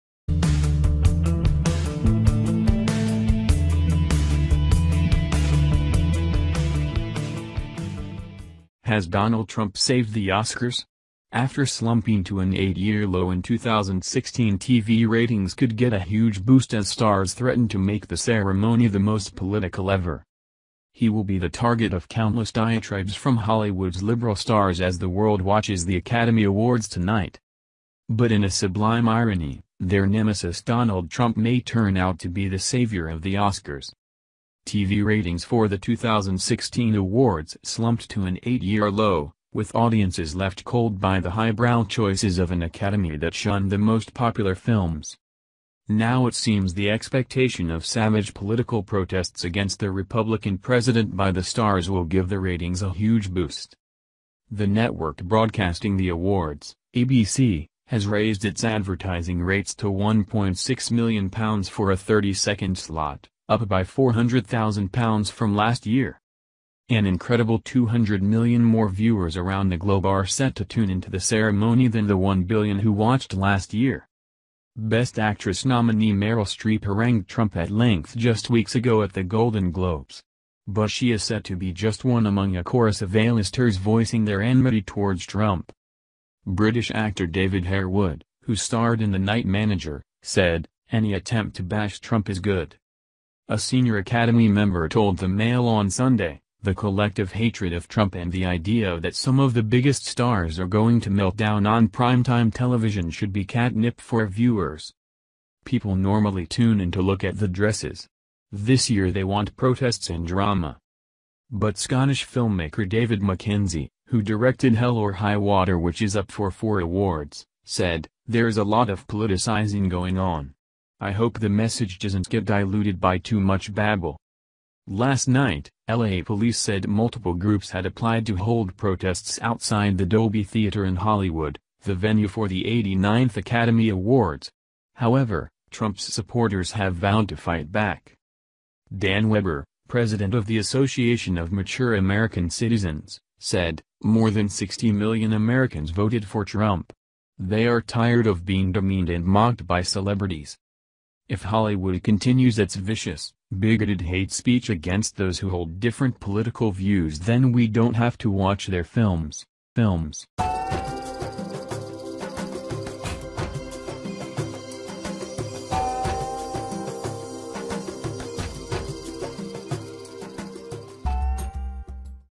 Has Donald Trump SAVED the Oscars? After slumping to an eight-year low in 2016 TV
He will be the target of countless diatribes from Hollywood’s liberal stars as the world watches the Academy Awards tonight.
But in a sublime irony, their nemesis Donald Trump may turn out to be the saviour of the Oscars.
TV ratings for the 2016 awards slumped to an eight-year low, with audiences left cold by the highbrow choices of an Academy that shunned the most popular films